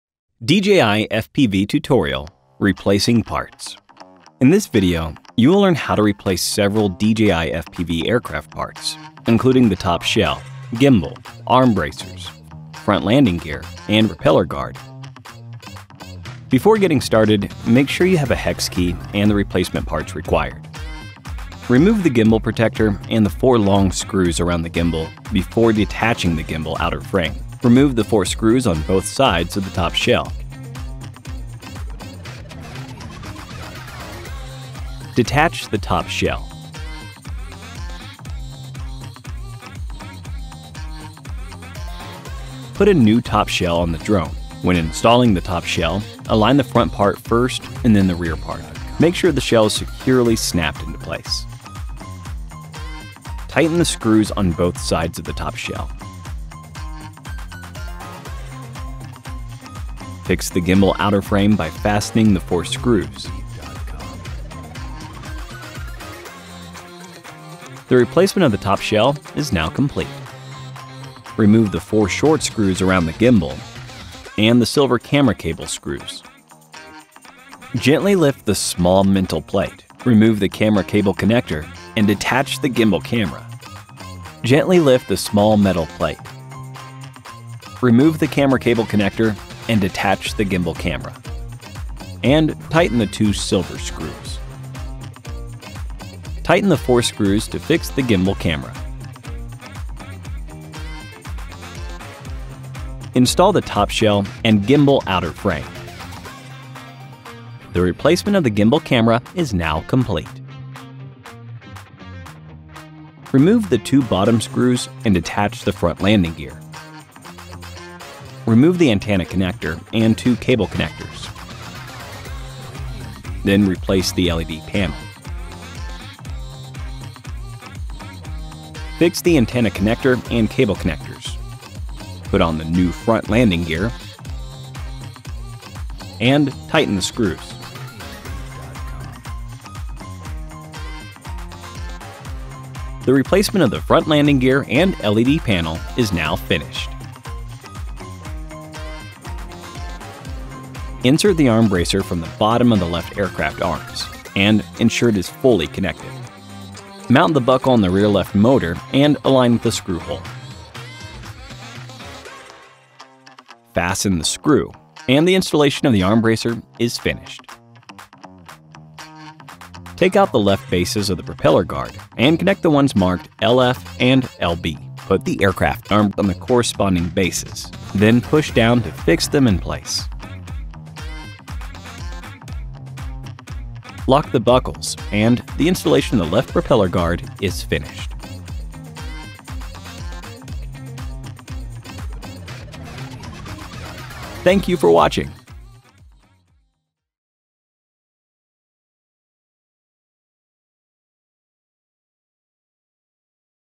DJI FPV | How to Easily Replace the Parts of DJI FPV
DJI DJIFPV Replacing Part
The gimbal camera, landing gear, and top shell of the DJI FPV aircraft are all modular and easily replaceable, making repairs more convenient. DJI Care Refresh is also available, offering coverage for a wide range of damage and giving you greater peace of mind as you fly.

This video will show you how to Easily Replace the Parts of DJI FPV !

Timecodes:
0:00 - Introduction
0:24 - How to Replace the Top Shell
1:13 - How to Replace the Gimbal Camera
1:55 - How to Replace Front Landing Gear
2:30 - How to Attach the Arm Bracers
2:53 - How to Attach the Propeller Guard